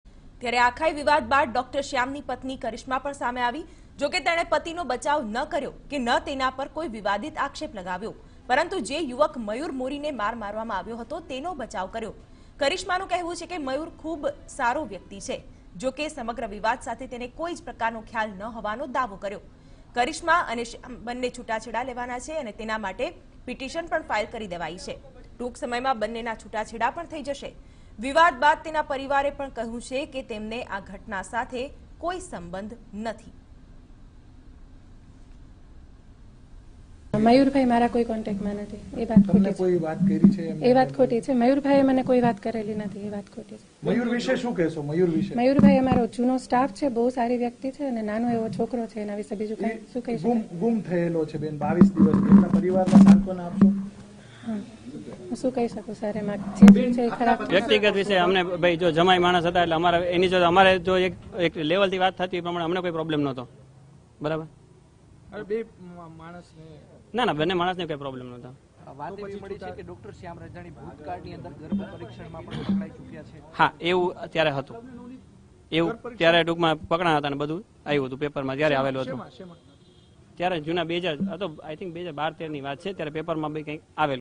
Rajkot: Case of Doctor thrashed a man inside a car; Doctor's wife refuse to give any statement- Tv9
Rajkot: Case of Doctor thrashed a man inside a car; Doctor's wife refuse to give any statement